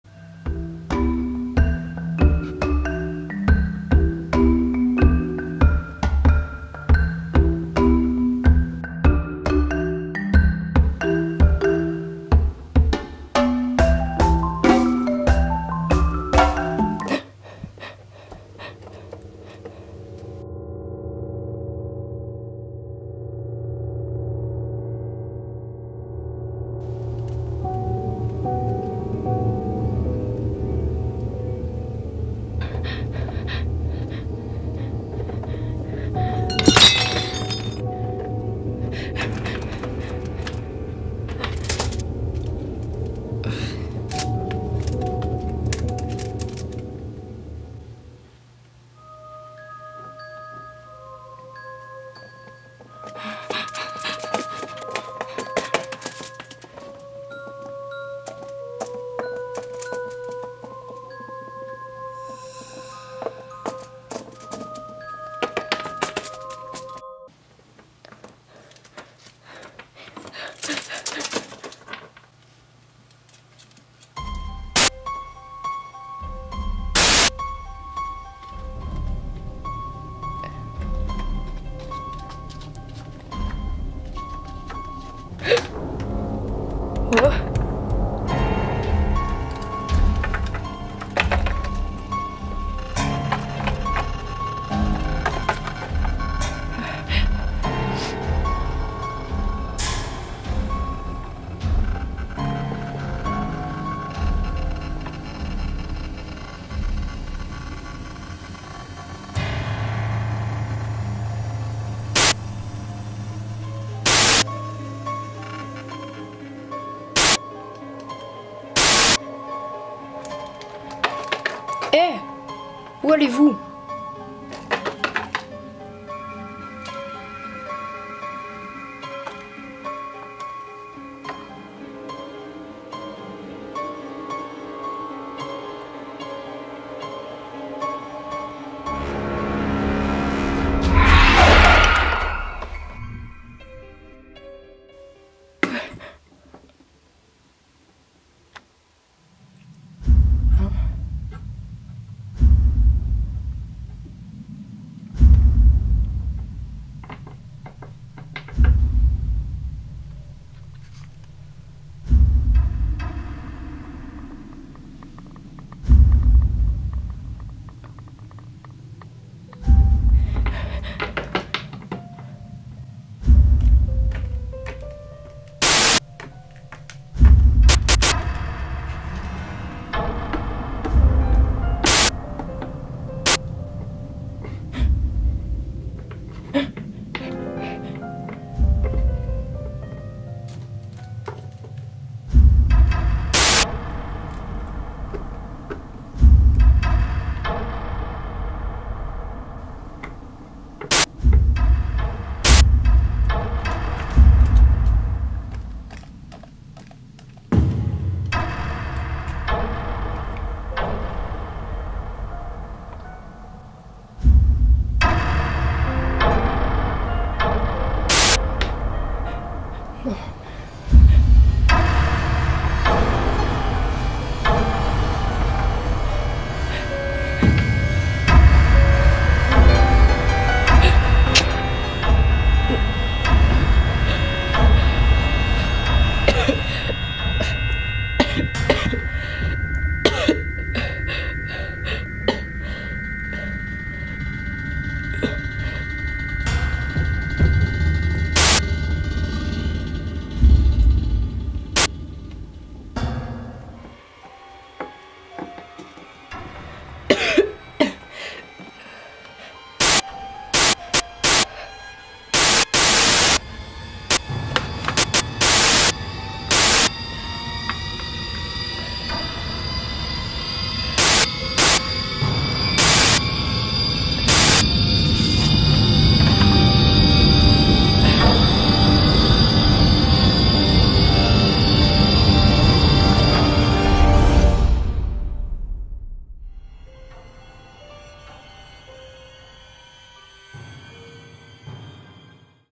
LPS: La Grande Illusion - [Court-Métrage] -10 ~FR
► HEYYYY :D Aujourd'hui on s'retrouve avec un nouveau court-métrage que j'ai fait assez rapidement parce que j'devais le rendre avant l'arrêt des notes pour mon projet d'arts visuels :'3 bref je sais que c'est un peu abstrait et tout mais si vous réfléchissez un peu vous comprendrez (fin j'espère xD) et donc la problématique c'était "Représenter l'illusion du réel" je sais pas si j'ai compris mais bon j'ai fait comme j'ai pensé j'espère que ça vous plaît :'D Bisous ♥

► Social Network ◄

► Personnages Principaux: Sans Nom.
► Temps: ~7h40
► Logiciel: Sony Vegas Pro 12.